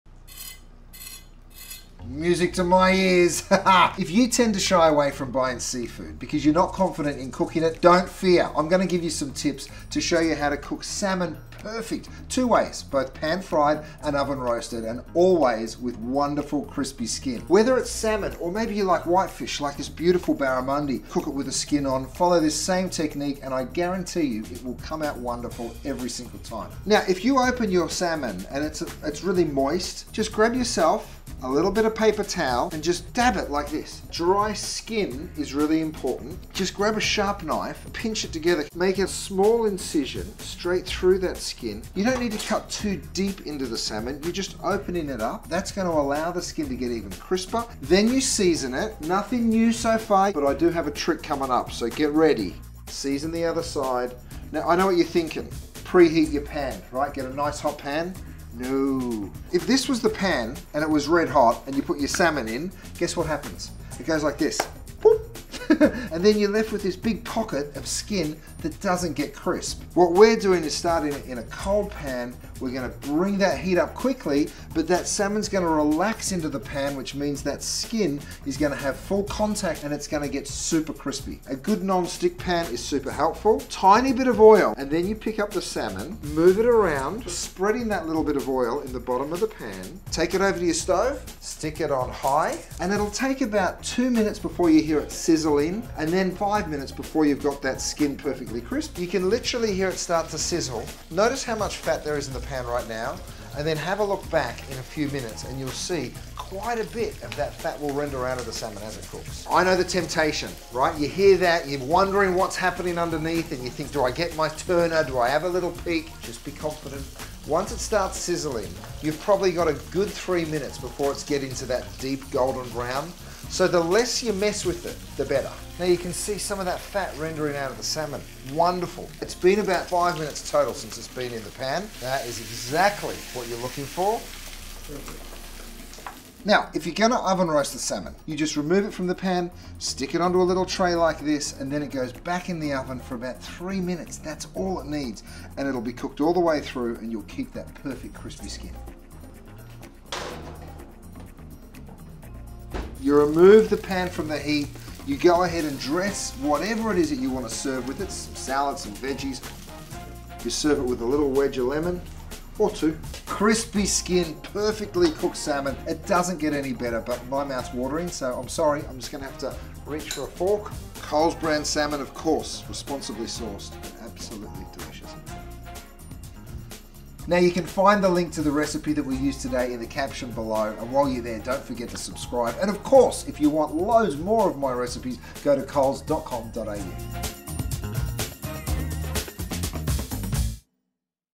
Curtis Stone’s crispy skin salmon | Cooking with Curtis Stone | Coles
Learn two delicious ways to cook salmon at home with Curtis Stone. Whether you prefer pan-fried salmon or oven-baked salmon, Curtis will teach you the tips and techniques to ensure a delicious crispy skin every time. Master these simple methods and impress yourself and your loved ones with a restaurant-quality crispy skin salmon dinner – in way less time than you thought!

Crispy-skin salmon
Serves 2 Cooking 5 mins Prep 10 mins

2 (about 120g each) salmon fillets, skin on
Olive oil spray
Sea salt flakes and freshly ground black pepper, to season

1. If you want the skin to crisp up in the pan, it needs to be completely dry before you start cooking. Place the salmon fillets skin-side up on a plate and gently pat the skin with paper towel.
2. A really hot pan is essential for getting the skin to go crispy, so preheat a frying pan on high. Spray the skin side with oil. Alternatively, spray the pan with oil before preheating.
3. Seasoning does more than add flavour – the salt also draws out moisture from the skin so it crisps up quickly. Sprinkle the skin with sea salt flakes and freshly ground black pepper.
4. Work quickly to crisp the skin without overcooking the salmon. Place skin-side down in pan and cook for 3 mins or until crispy. Turn and cook for 2 mins or until cooked to your liking.

00:00 - Intro
00:26 - How to dry
00:36 - How to score
00:48 - How to season
01:10 - How to pan fry salmon
01:30 - Add pan to stove
01:57 - Flip the salmon
02:16 - How to oven roast salmon
02:34 - How to serve
02:44 - Final dish